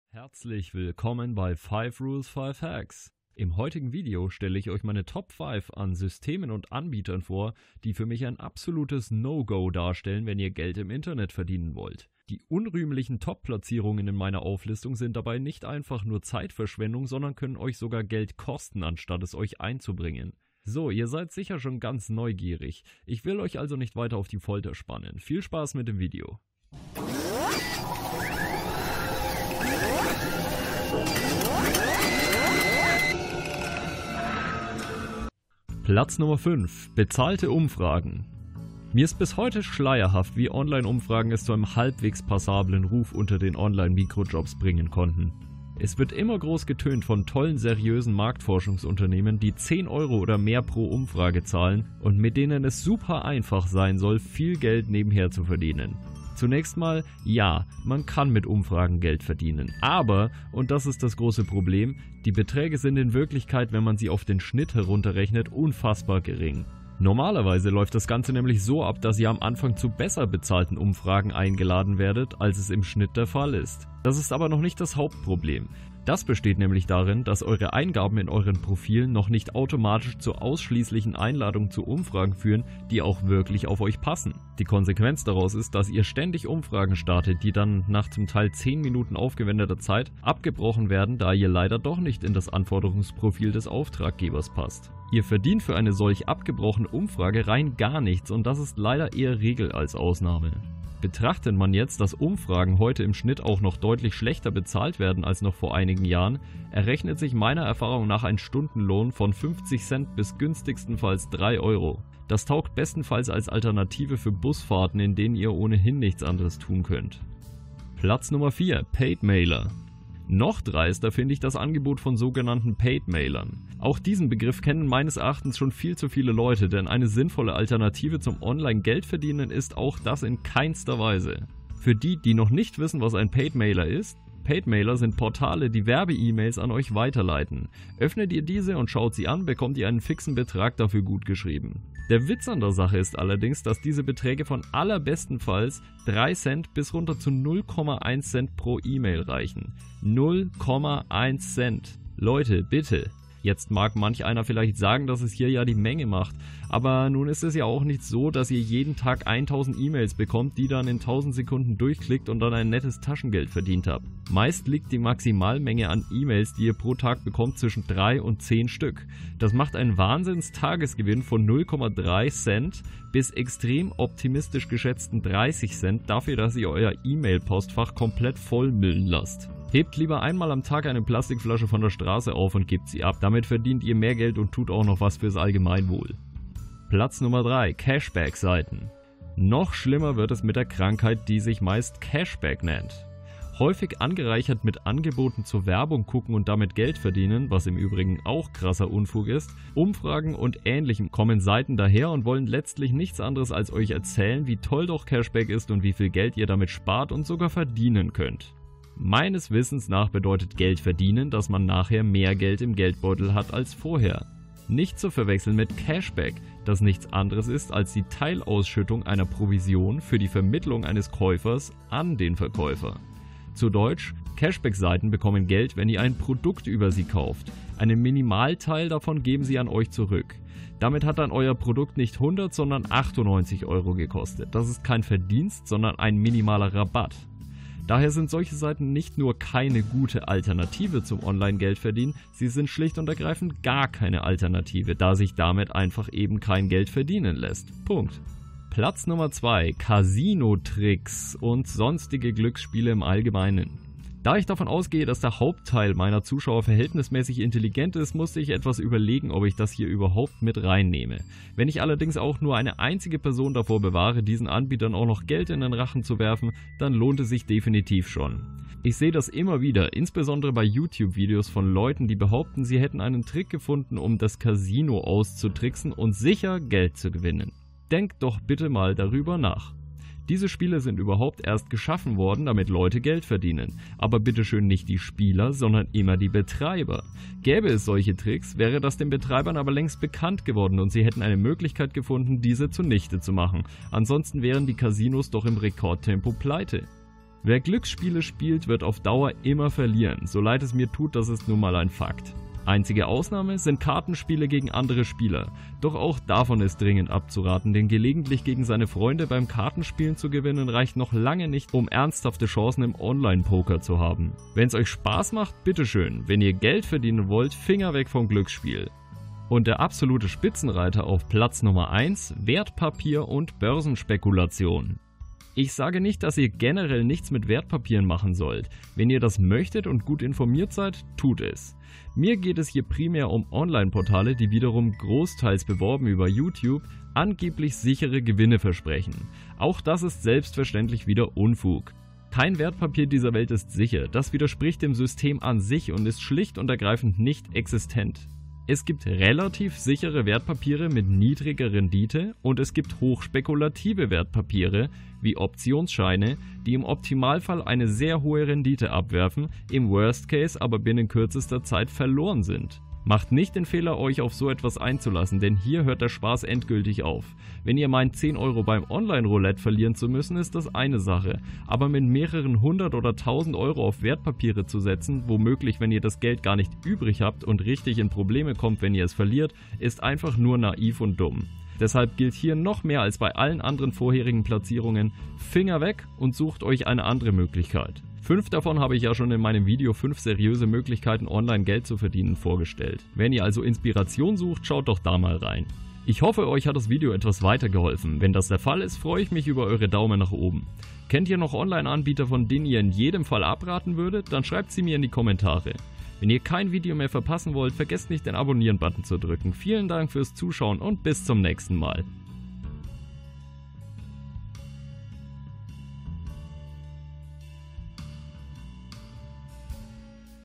Seriös online Geld verdienen? - Die 5 größten No-Gos! 5Rules5Hacks
SERIÖS online Geld verdienen. Sei es um sein Taschengeld aufzubessern, sei es weil man wirklich dringend zusätzliches Geld braucht um über die Runden zu kommen. Das ist etwas, was viele von uns gerne möchten. Doch oft sieht die Realität anders aus.
In diesem Video stelle ich euch meine persönlichen No-Gos bezüglich des online Geld verdienens vor.
Das Angebot der schwarzen Schafe reicht dabei von Zeitverschwendung bis hin zur Gefährdung des Geldes, das ihr schon besitzt.
Also Finger weg von DIESEN 5 Angeboten!

HILFREICHE BÜCHER ZUM THEMA:

"45 Wege zum online Geld verdienen - mit oder ohne Website"